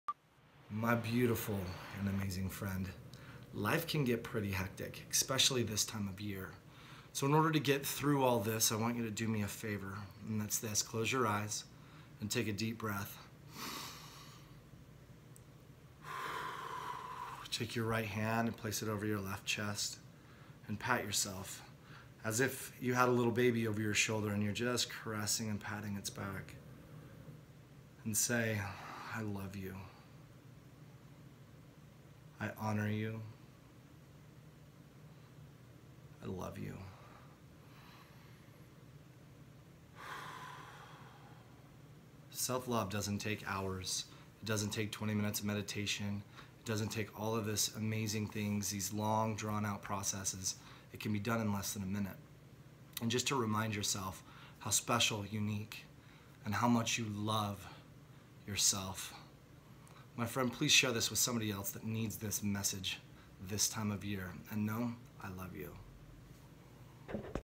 Mediation in a Minute
My friend, life can get pretty hectic this time of year, and it can feel overwhelming.

Take one minute out of your day to learn this tactic that can make you feel loved and release mountains of stress.

Then please share this gift with someone you feel could use this.

Love,
Greg